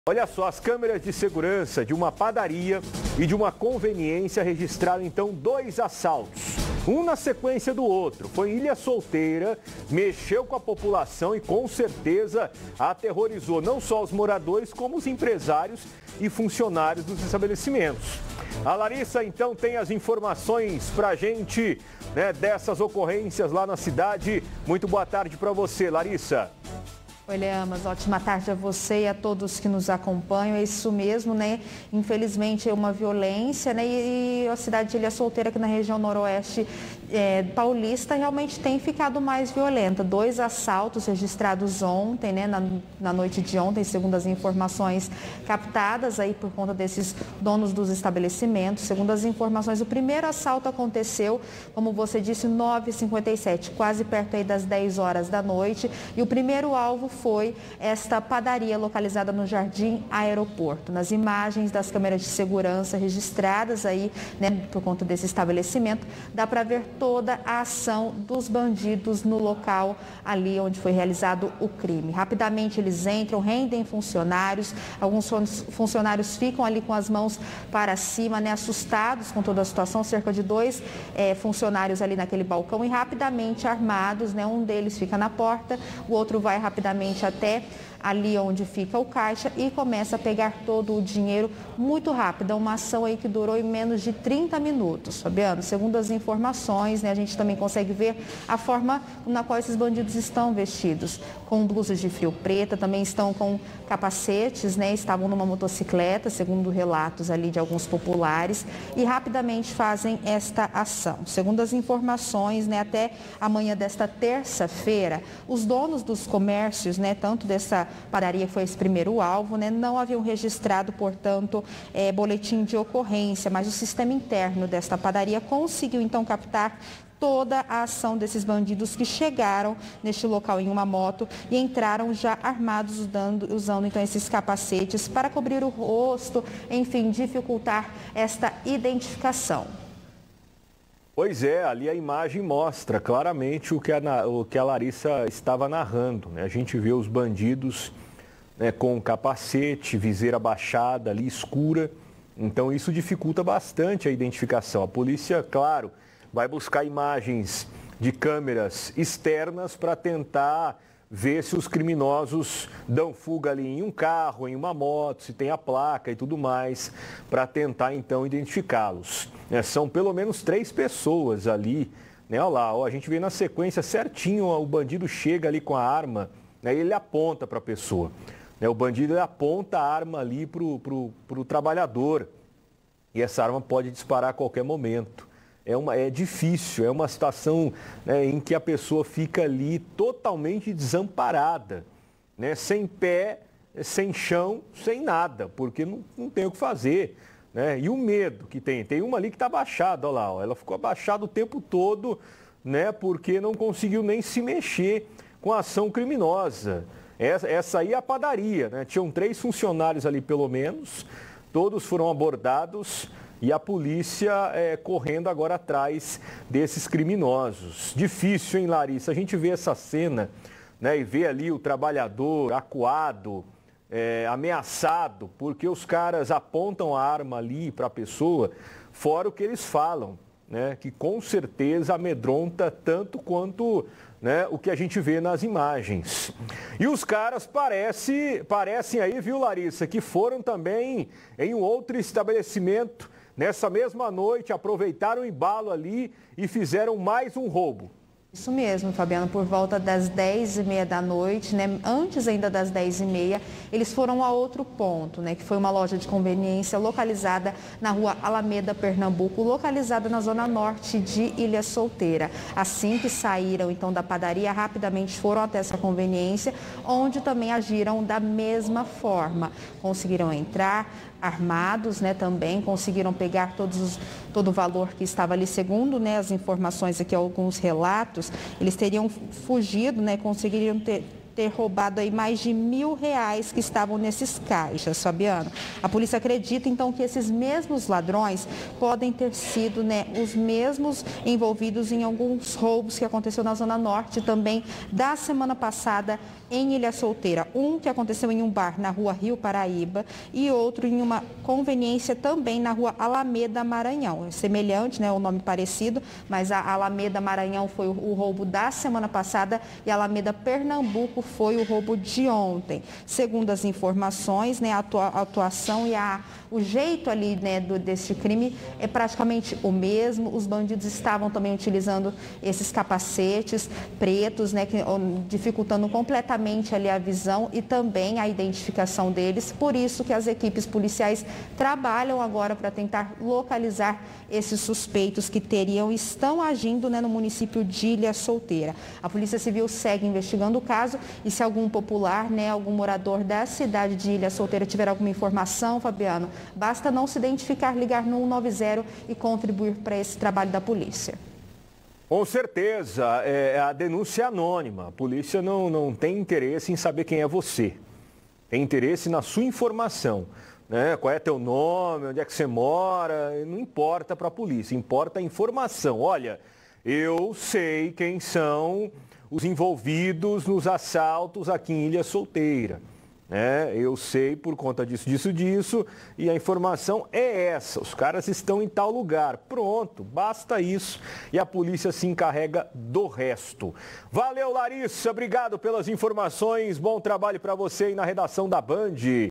Sequência de assaltos aterroriza moradores em Ilha Solteira
As câmeras de segurança de uma padaria e de uma conveniência registraram dois assaltos: um na sequência do outro em Ilha Solteira (SP).

Mande seu alô, seu abraço, sua opinião, sua mensagem através do Bandzap: (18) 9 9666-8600